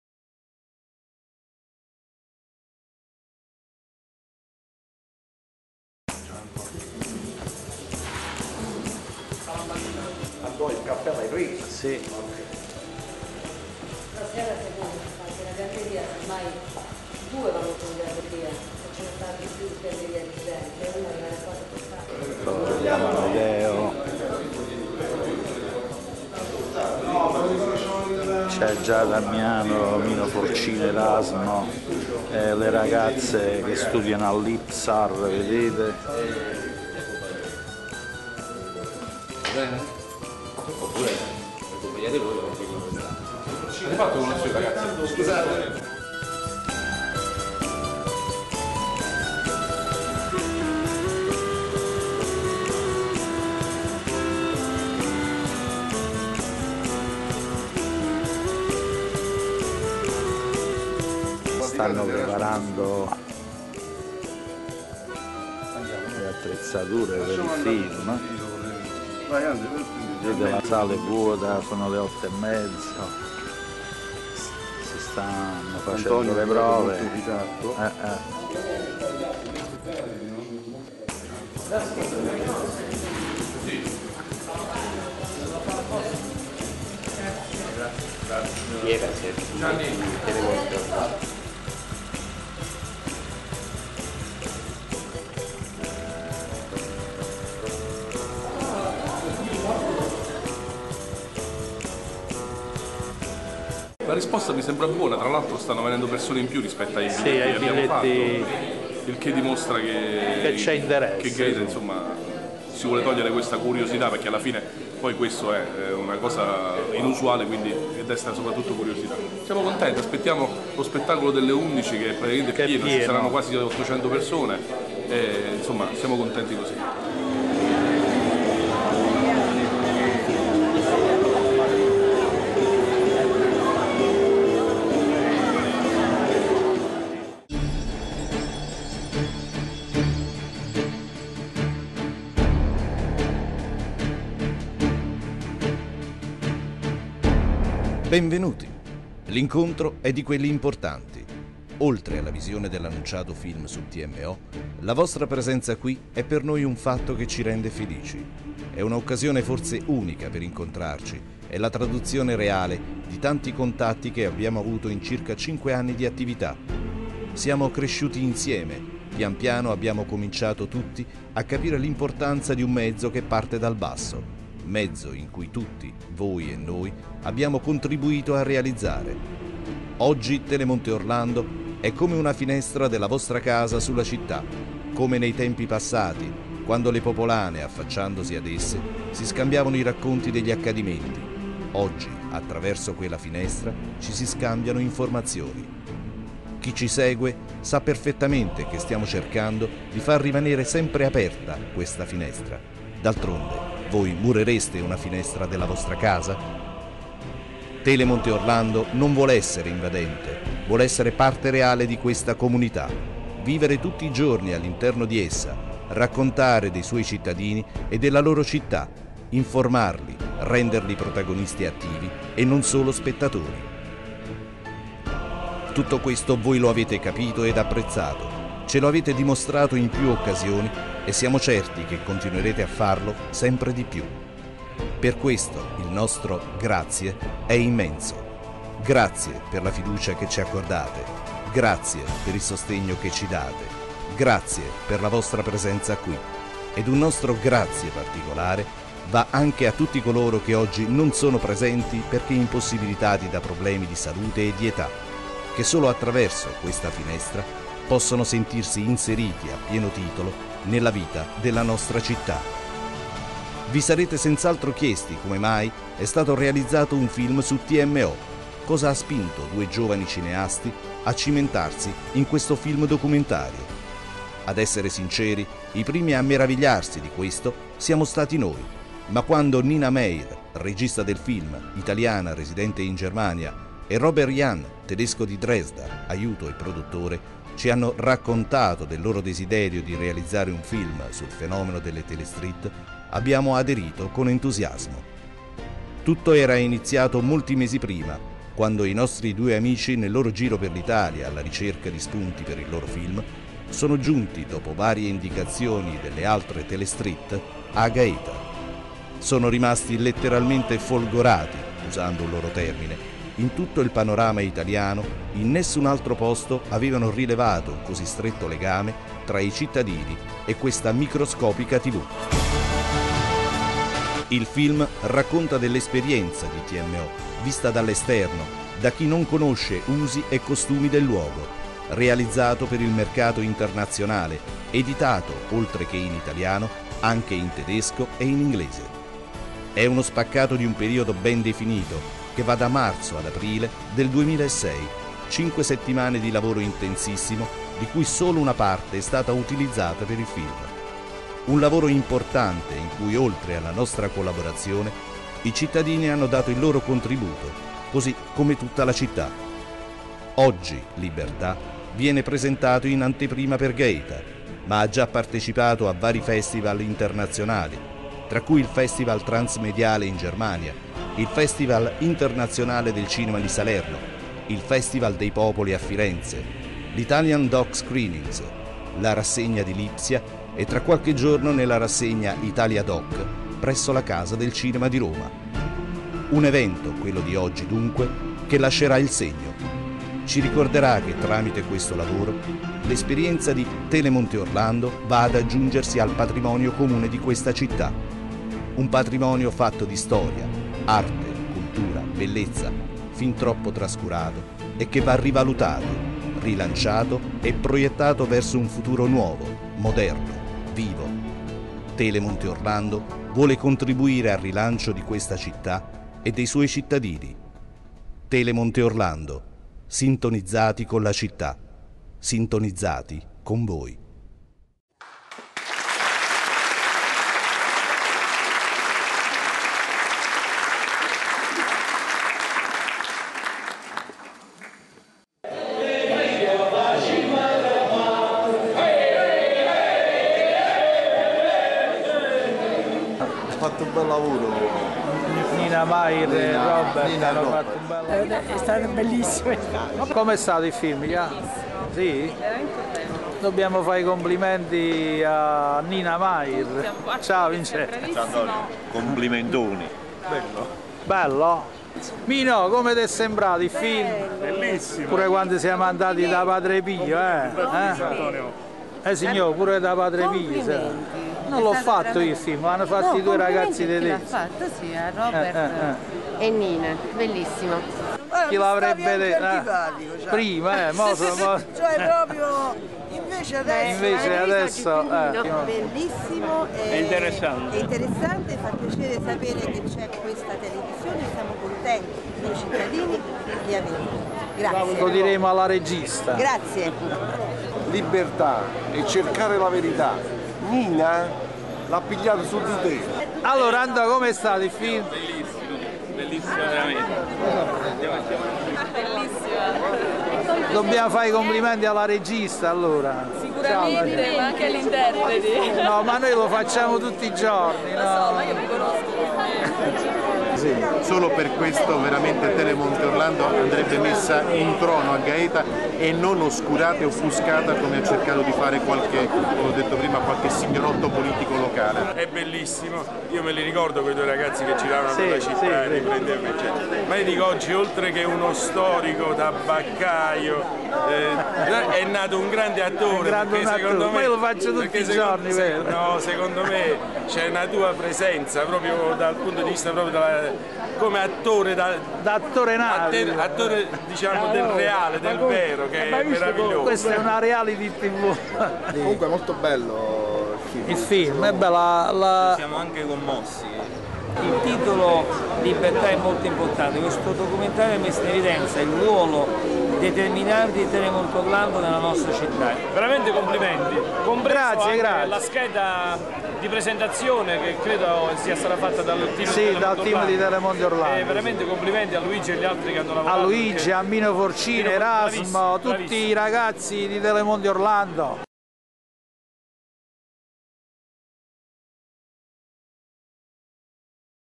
LIBERTA', IL FILM SU TMO GAETA
11 Marzo 2007, Cinema Ariston, Gaeta. La prima Nazionale del film su TMO TeleMonteOrlando Gaeta raduna quasi 2000 persone! In questo video il racconto dell'indimenticabile giornata vissuta dagli "Orlandones" e da tutta la Città.